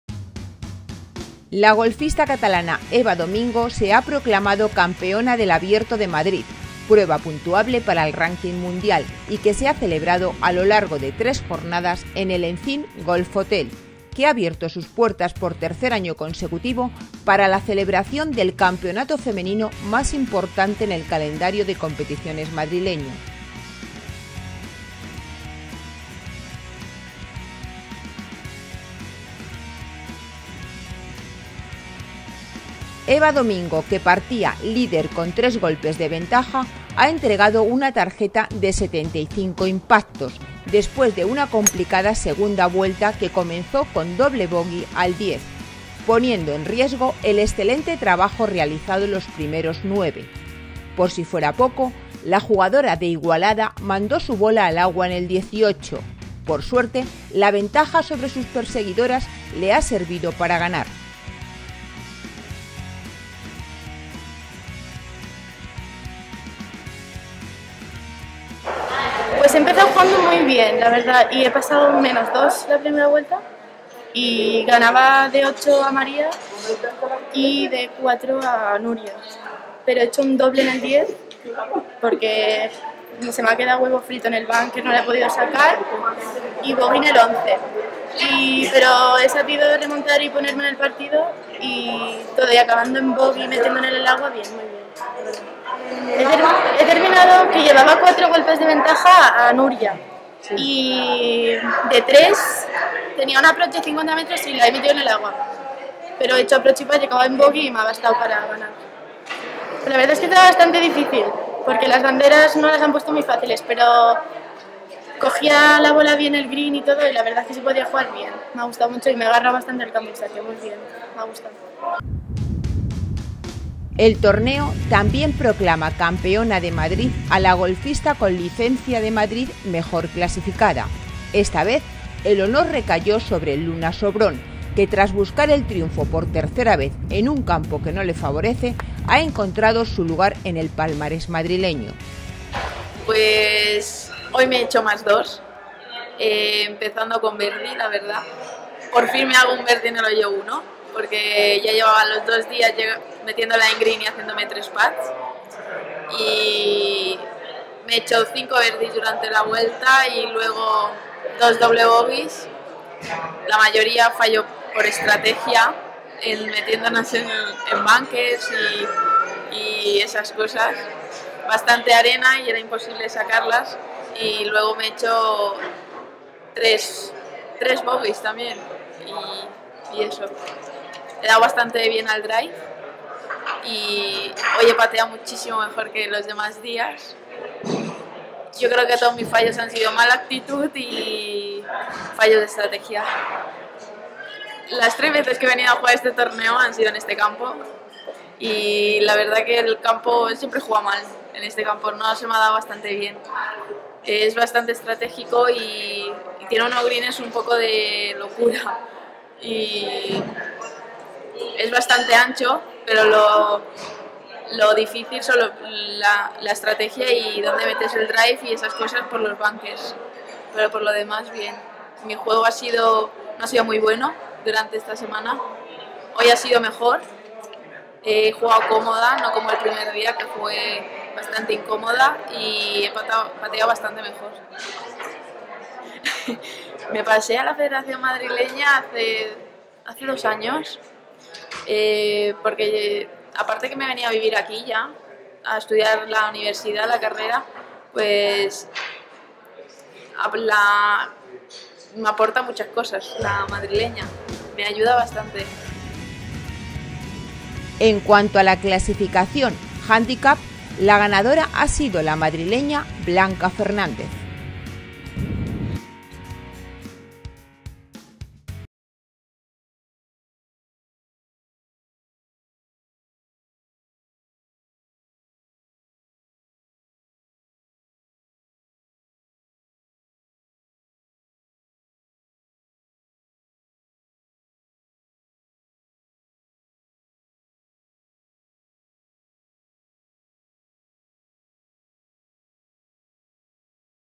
Campeonato Abierto de Madrid Femenino (y III)
La golfista catalana Eva Domingo se ha proclamado Campeona del Abierto de Madrid, prueba puntuable para el Ranking Mundial, y que se ha celebrado a lo largo de tres jornadas en El Encín Golf Hotel, que ha abierto sus puertas por tercer año consecutivo para la celebración del Campeonato femenino más importante en el calendario de competiciones madrileño.